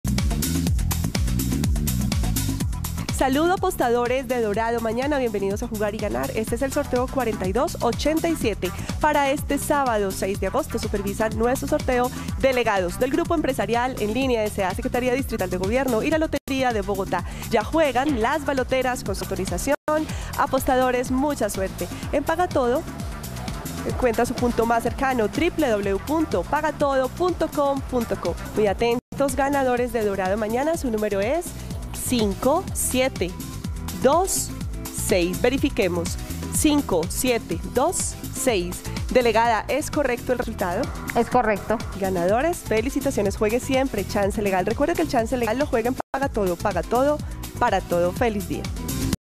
EL DORADO MAÑANA Resultado del SÁBADO 06 DE AGOSTO de 2022 ✅🥇🔥💰
Hola buenos días, te presentamos los resultados de las loterías y chances en Colombia, vamos con el Resultados del Chance Dorado mañana del SÁBADO 06 DE AGOSTO de 2022.